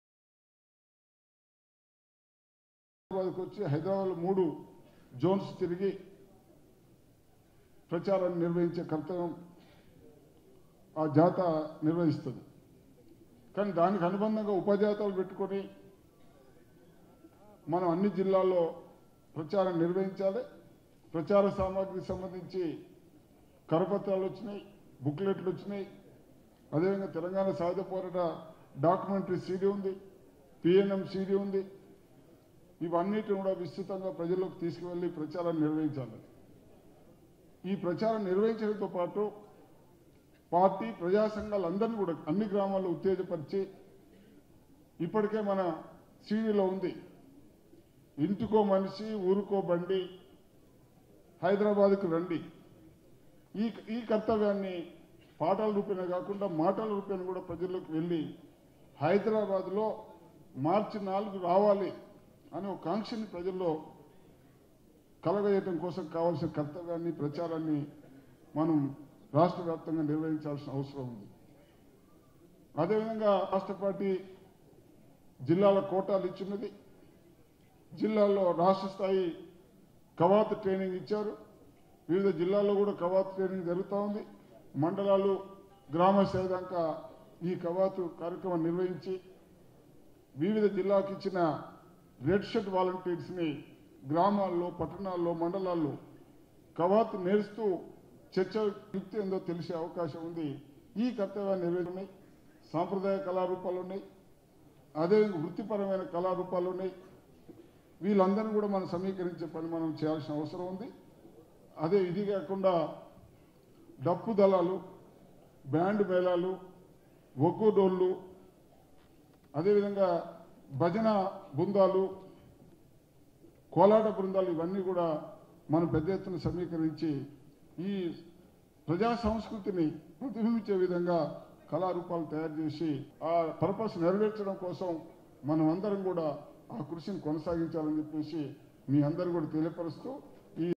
Nandyala Narsimhareddy About "INTINTIKI CPIM"Campaign
Nandyala Narsimhareddy About "INTINTIKI CPIM"Campaign  at ravindra bharathi